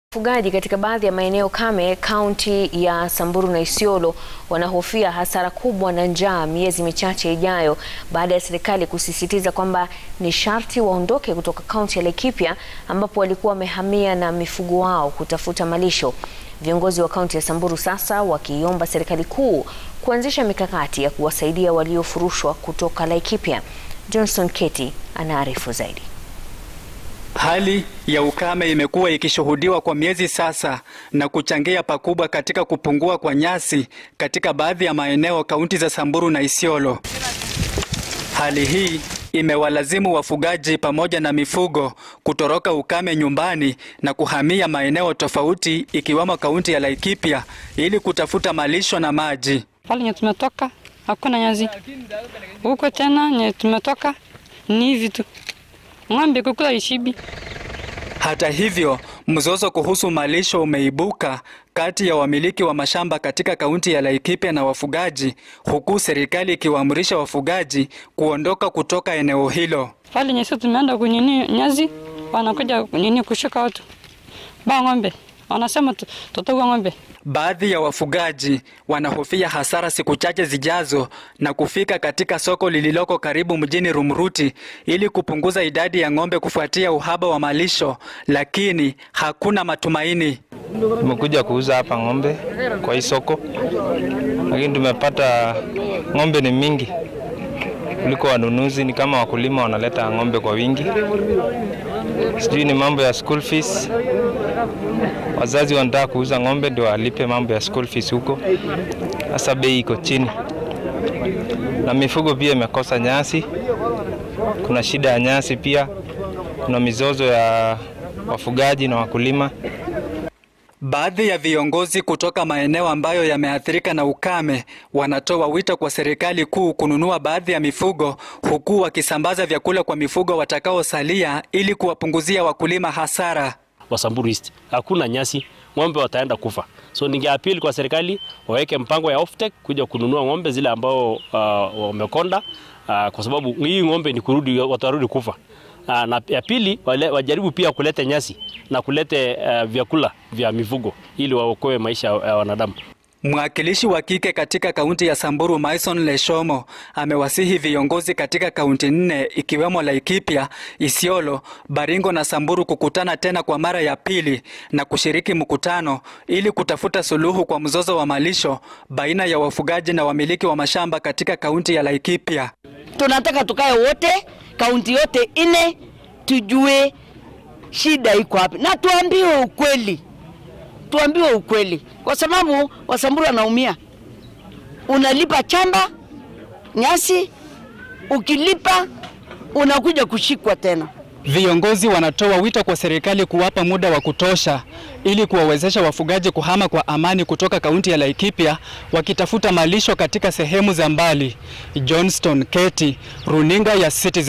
Wafugaji kaunti za Samburu na Isiolo aanalalamikia kuhangaikia maisha baada ya kufurushwa Laikipia
Wafugaji katika baadhi ya maeneo kame kaunti ya Samburu na Isiolo wanahofia hasara kubwa na njaa miezi michache ijayo baada ya serikali kusisitiza kwamba ni sharti waondoke kutoka kaunti ya Laikipia ambapo walikuwa wamehamia na mifugo wao kutafuta malisho. Viongozi wa kaunti ya Samburu sasa wakiiomba serikali kuu kuanzisha mikakati ya kuwasaidia waliofurushwa kutoka Laikipia.